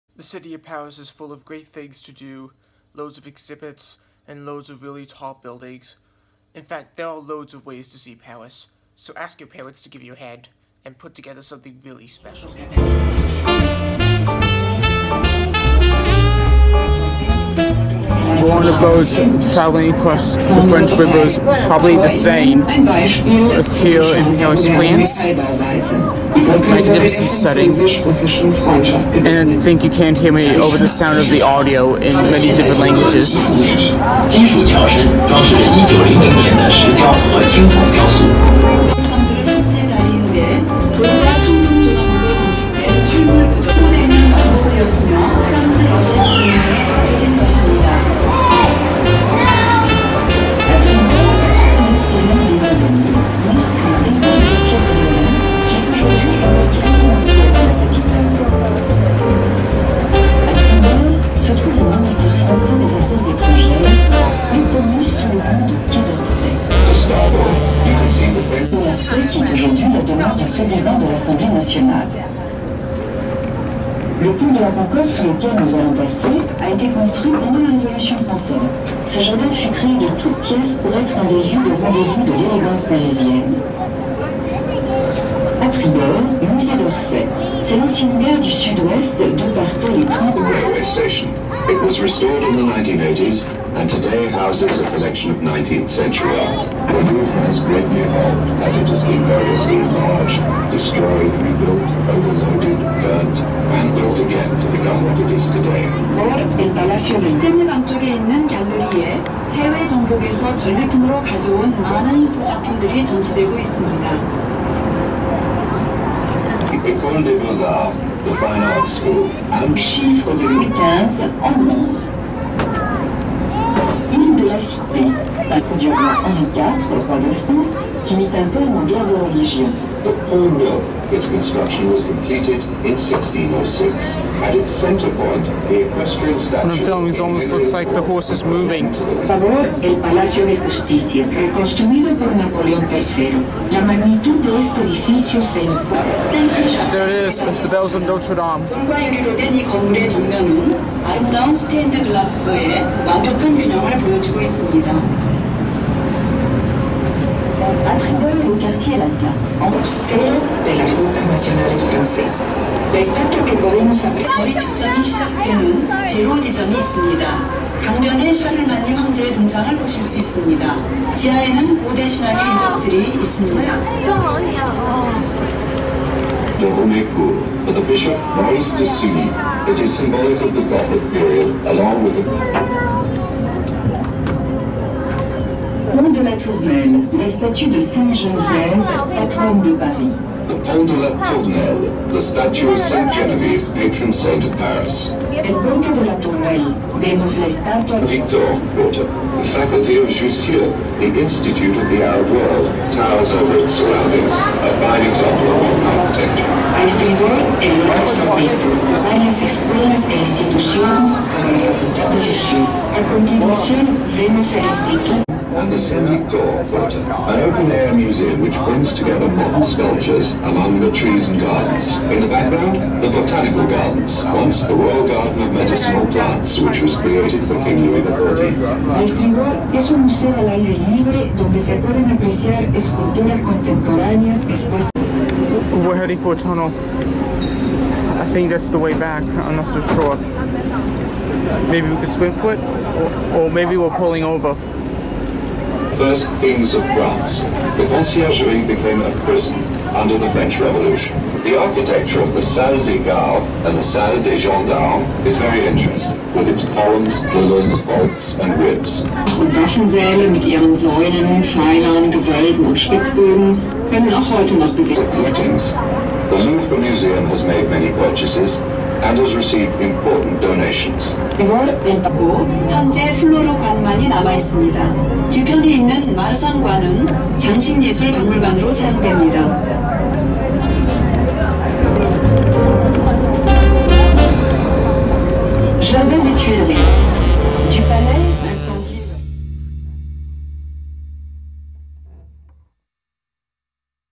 The Final Days in Paris...
This is a video I put together on iMovie with unseen footage from my trip to DisneylandParis. On the final day of the Paris, France trip, my young broski, my mother and I went on a boat tour of the streets of Paris and saw some of the city's famous exhibitions and buildings.

Don't forget to talk to me in the community tab on YouTube…